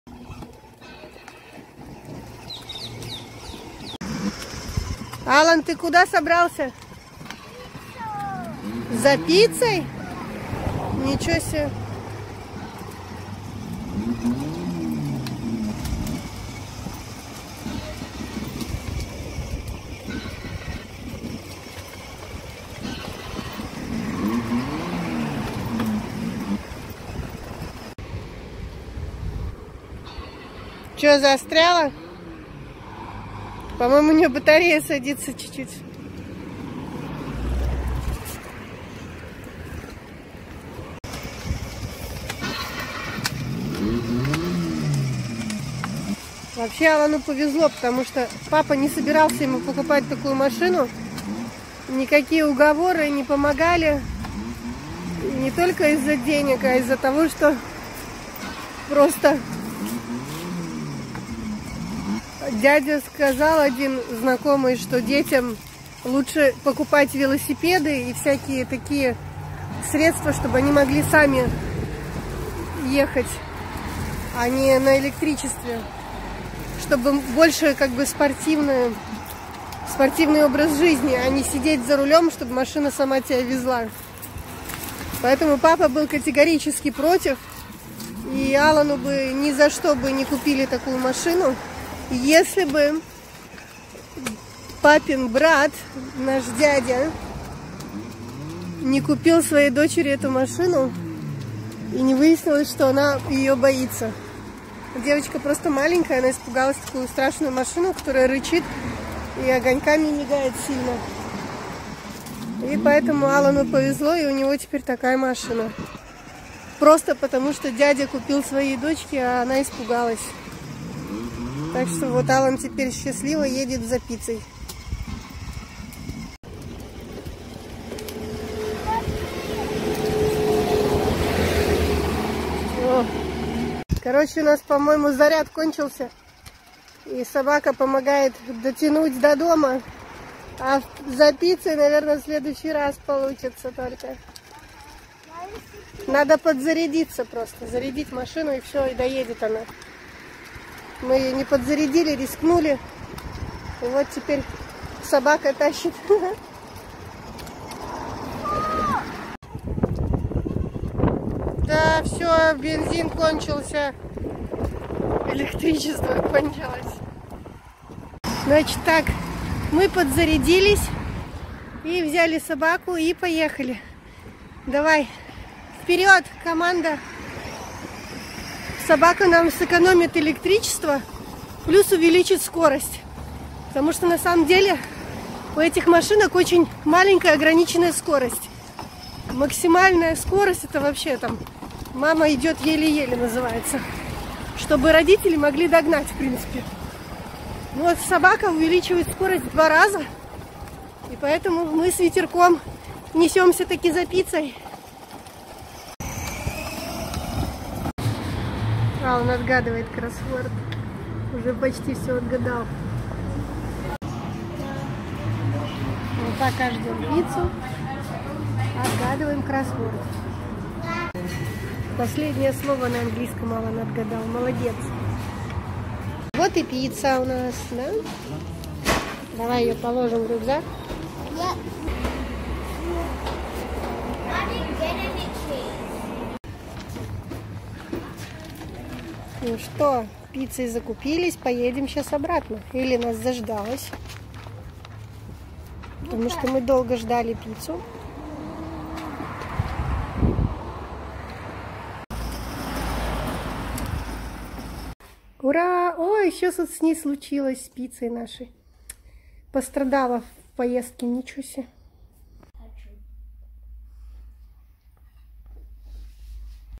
Еле Доехали за Пиццей - Собака помогла!
Папа не собирался покупать такую машину, тк считал, что лучше велосипеды и самокаты - более спортивные способы передвижения. Но папин брат купил дочке машину, а она ее испугалась и наотрез отказалась ездить. Вот так и сбылась мечта Алана о своей электрической машине.

В поход за пиццей на машине с собакой. По пути у машины села батарея и пришлось возвращаться, чтобы зарядить аккумулятор... Собака тянула машину
Долгое путешествие на машине в ближайшую пиццерию
Едем за пиццей.